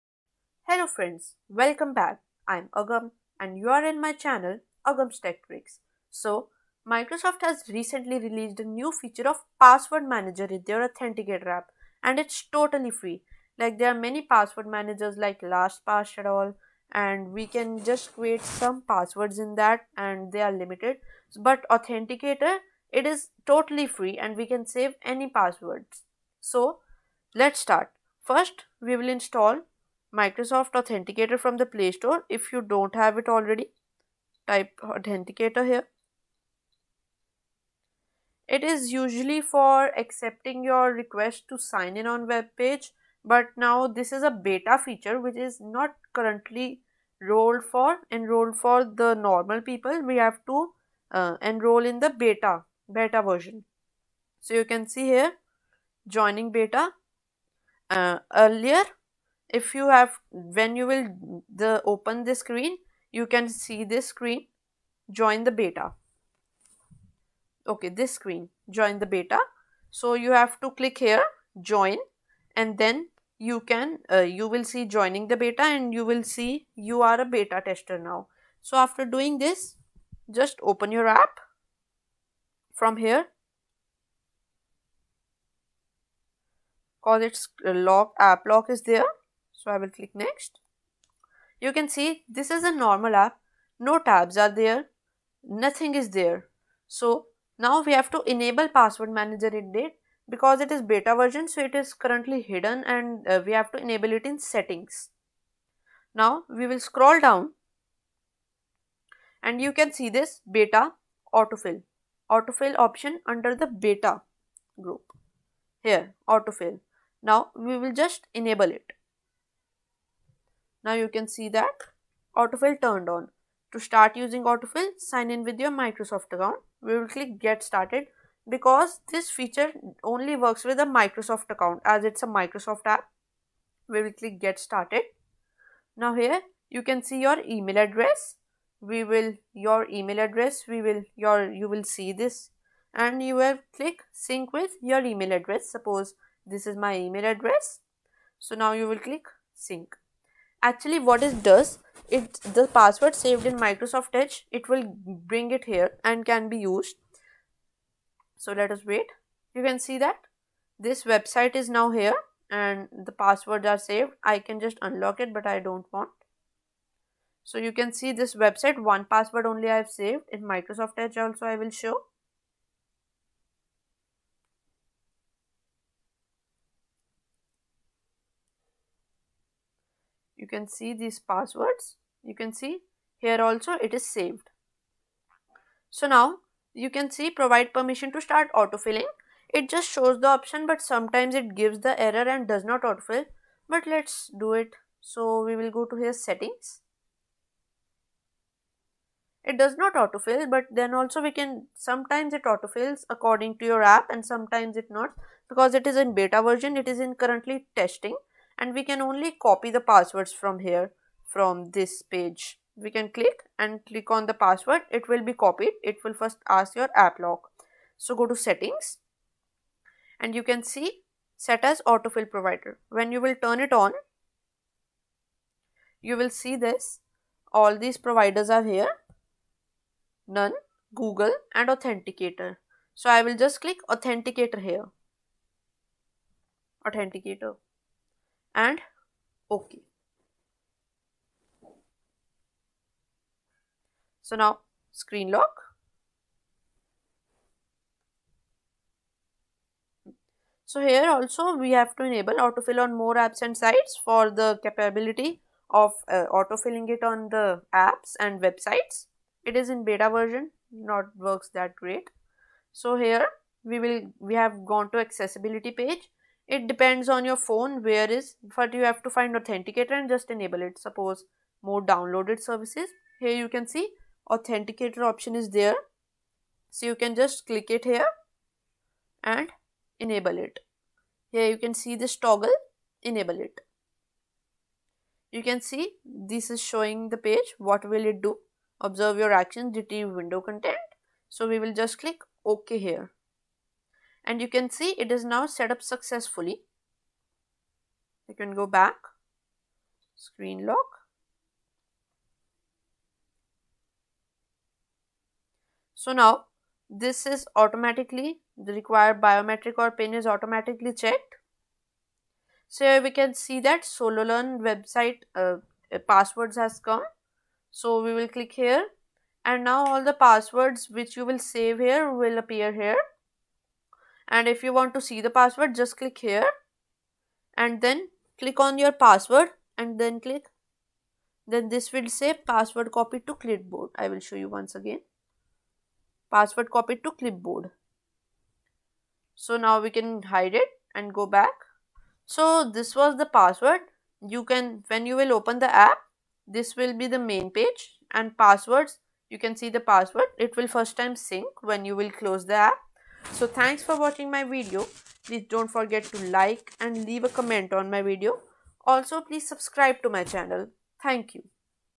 Use Microsoft Authenticator as Password Manager | Totally Free!!
Join me on Telegram - @agamtechtricks

Hello Guys 👋,

Want a password Managers that is free of cost. Here I am. Microsoft has released a new feature of Password Manager in their app. (BETA Version). All the instructions are there in the video.